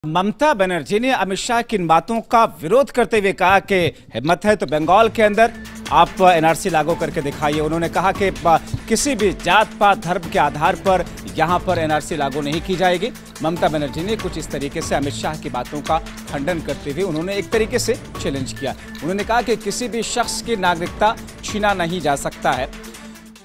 NRC पर Mamata Banerjee का AmitShah पर पलटवार
पश्चिम बंगाल की मुख्यमंत्री ममता बनर्जी ने एनआरसी के मुद्दे पर केंद्रीय गृह मंत्री अमित शाह पर पलटवार किया है...ममता बनर्जी ने कहा कि हम एनआरसी को बंगाल में नहीं लागू होने देंगे...